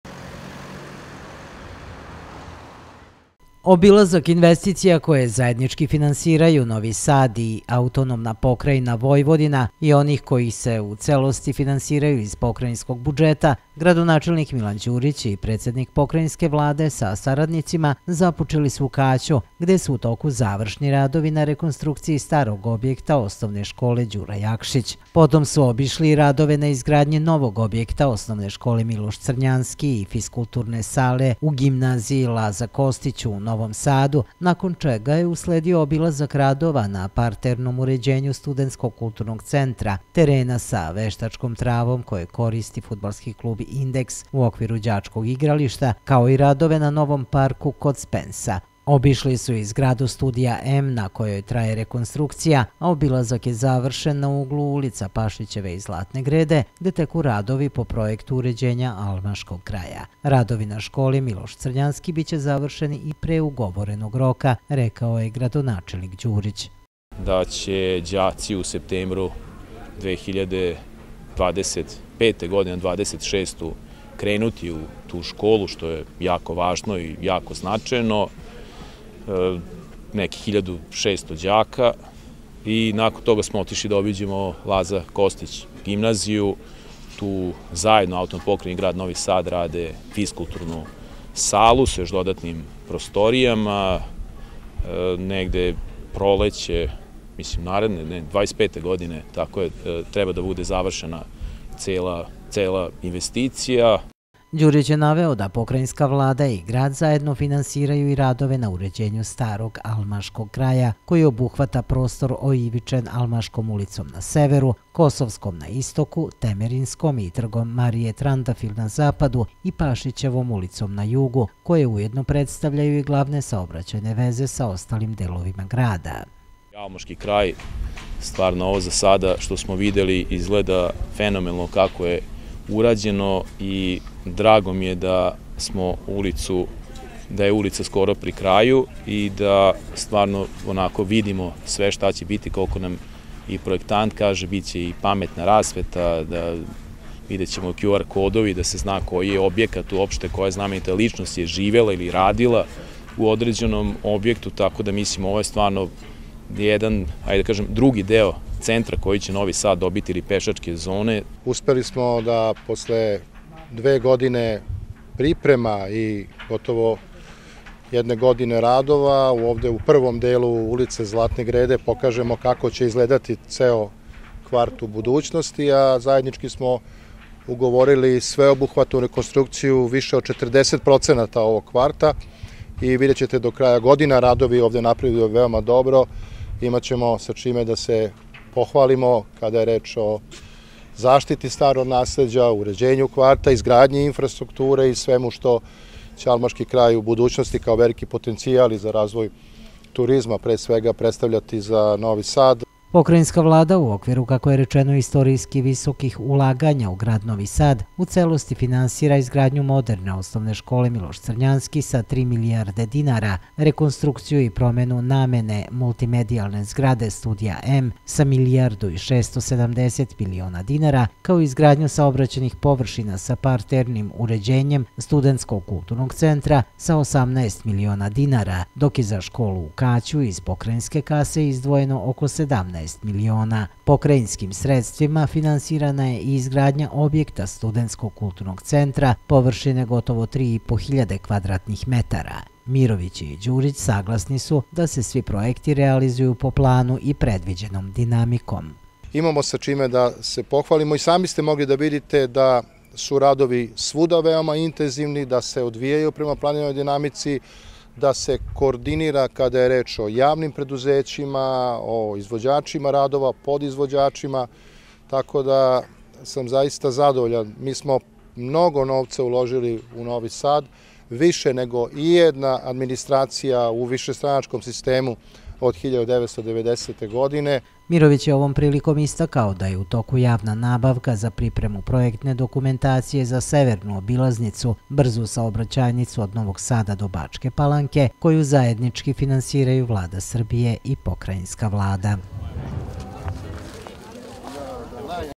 TV KANAL 9, NOVI SAD: ZNAČAJNA ULAGANJA POKRAJINE U NOVI SAD
Na području Novog Sada u toku su brojni radovi koje zajednički finansiraju grad i Pokrajina, kao i oni koji se iz u potpunosti finansiraju iz pokrajinskog budžeta. Gradilišta su danas obišli Igor Mirović, predsednik Pokrajinske vlade i gradonačelnik Milan Đurić.